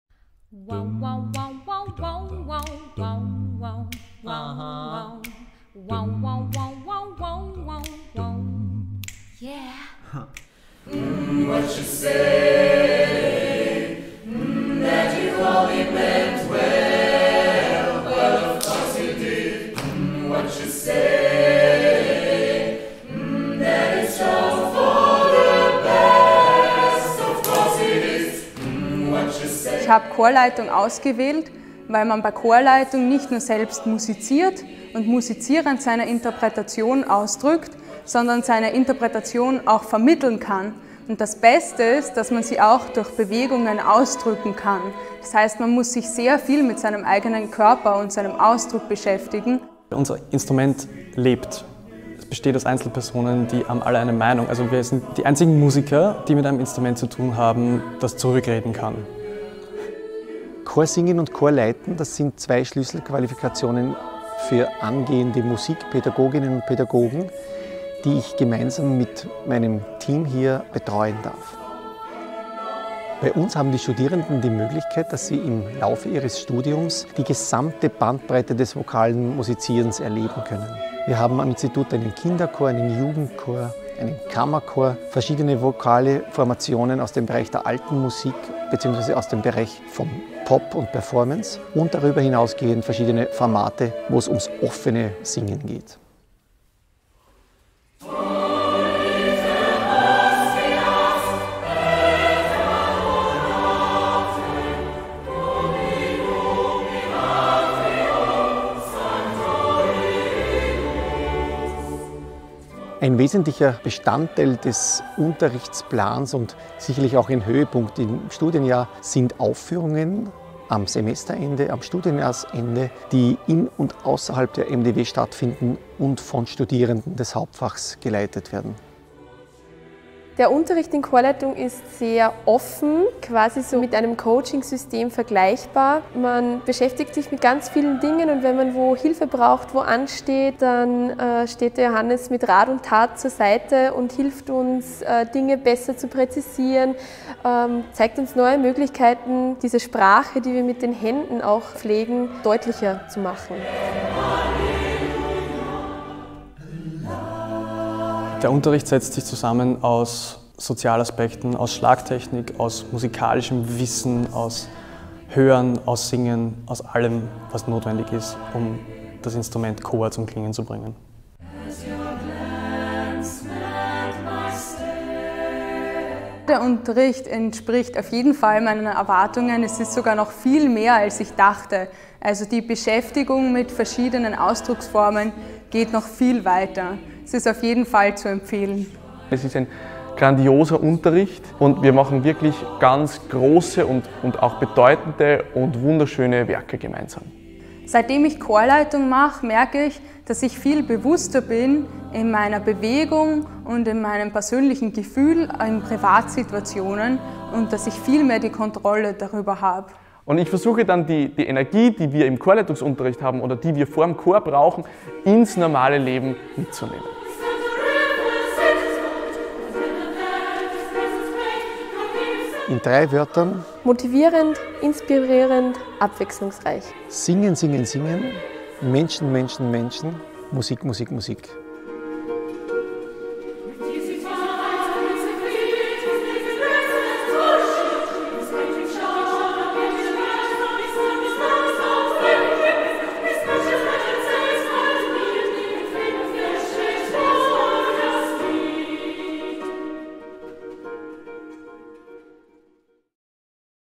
Imagevideo - Hauptfach Chorleitung
An der mdw – Universität für Musik und darstellende Kunst Wien gibt es im Rahmen des Musikerziehungsstudiums sowie des Instrumentalmusikerziehungsstudiums die Möglichkeit, Chorleitung als künstlerisches Hauptfach zu wählen.